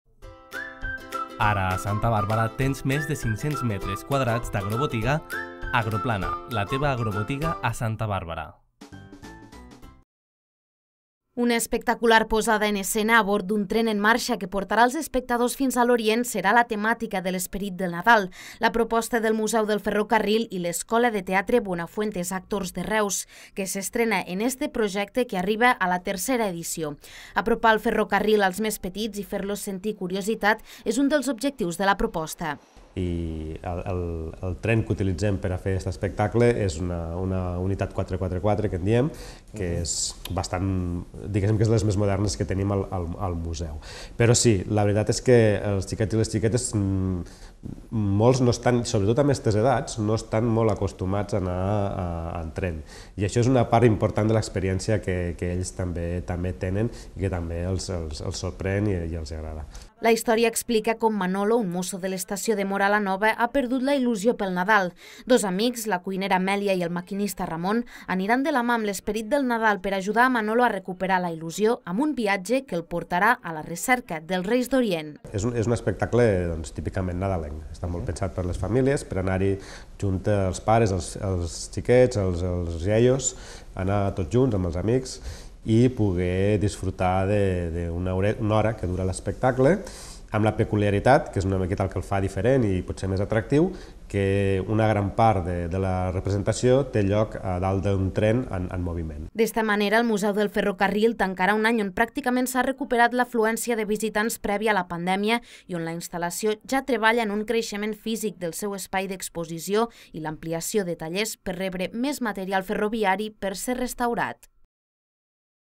El Museu del Ferrocarril de Móra la Nova recupera l'esperit per Nadal
Una espectacular posada en escena a bord d'un tren en marxa que portarà als espectadors fins a l'Orient serà la temàtica de l'Esperit del Nadal, la proposta del Museu del Ferrocarril i l'escola de teatre Buenafuente's Actors de Reus, que s'estrena en este projecte que arriba a la tercera edició. Apropar el ferrocarril als més petits i fer-los sentir curiositat, és un dels objectius de la proposta.

La història explica com Manolo, un mosso de l'estació de Móra la Nova, ha perdut la il·lusió per Nadal. Dos amics, la cuinera Amèlia i el maquinista Ramon aniran de la mà amb l'Esperit del Nadal per ajudar a Manolo a recuperar la il·lusió, amb un viatge que el portarà a la recerca dels Reis d'Orient.

D'esta manera el Museu del Ferrocarril tancarà un any on pràcticament s'ha recuperat l'afluència de visitants prèvia a la pandèmia, i on la instal·lació ja treballa en un creixement físic del seu espai d'exposició i l'ampliació de tallers per rebre més material ferroviari per ser restaurat.